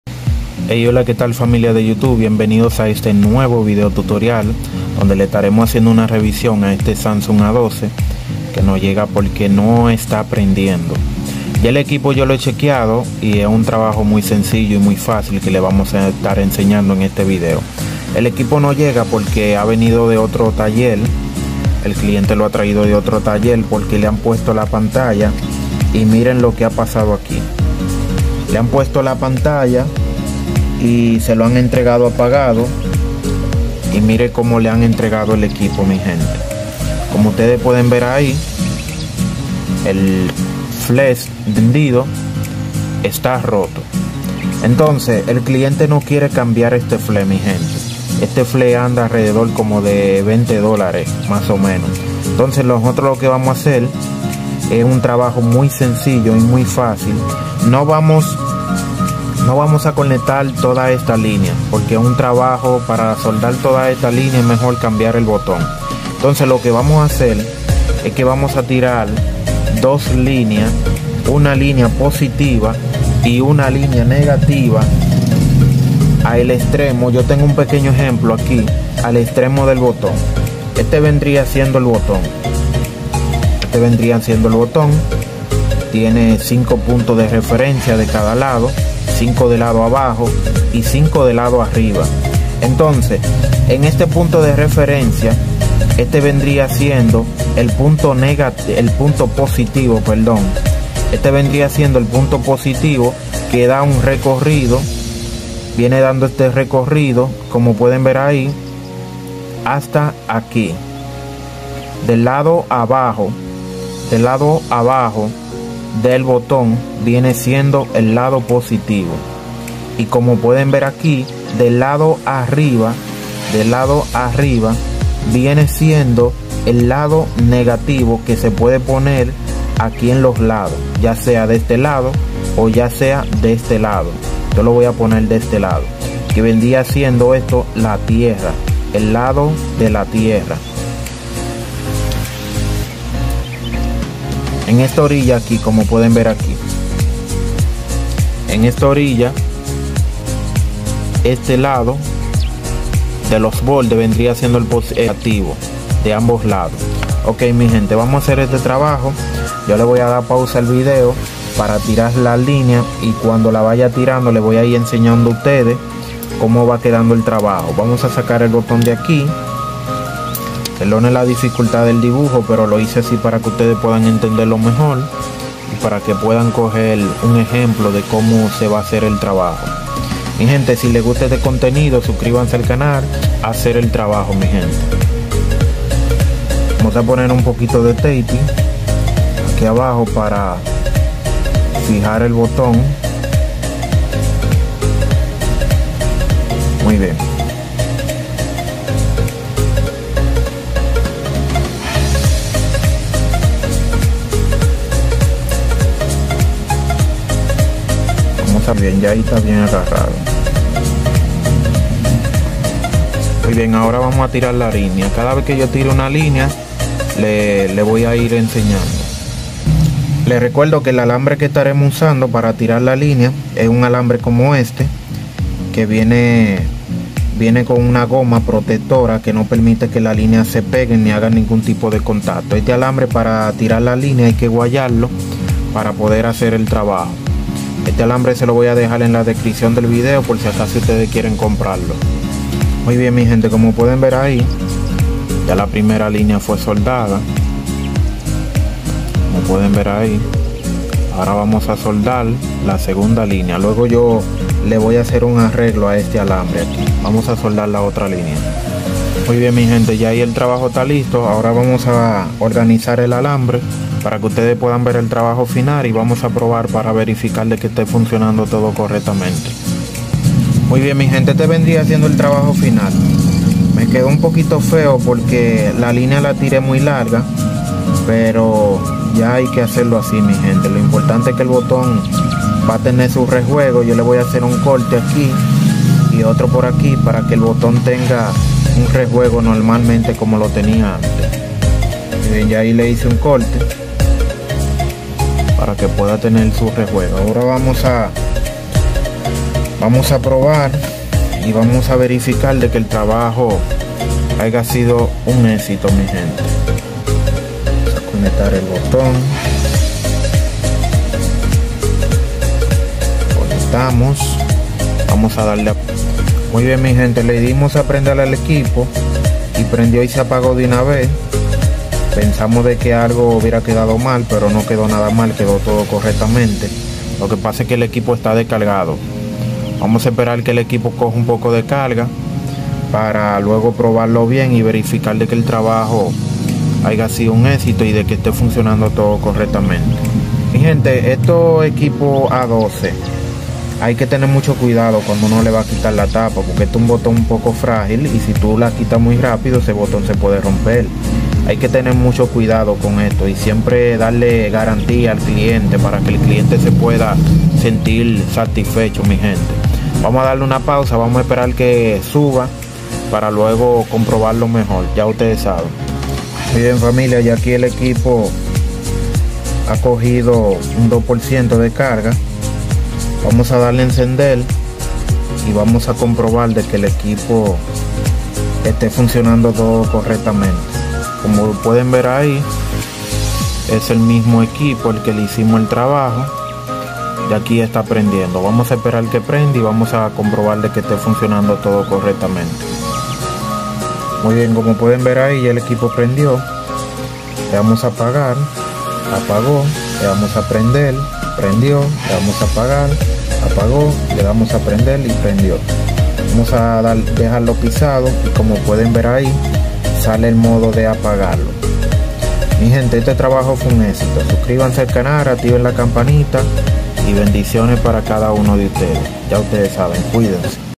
SAMSUNG A12 NO ENCIENDE REPARACIÓN Y SOLUCIÓN 2023
Como reparar el botón de encendido de un Samsung A12

SAMSUNG J337V FALLA DE IMAGEN SOLUCIÓN👨‍🎓🗣 alambre de cobre 0.004 in enlace